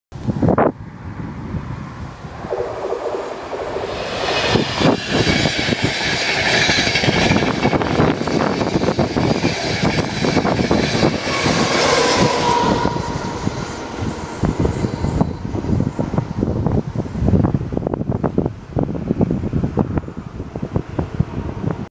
EastCoast 225 @ Grantshouse
EastCoast 225 @ Grantshouse working the 14:00 Edinburgh-London Kings X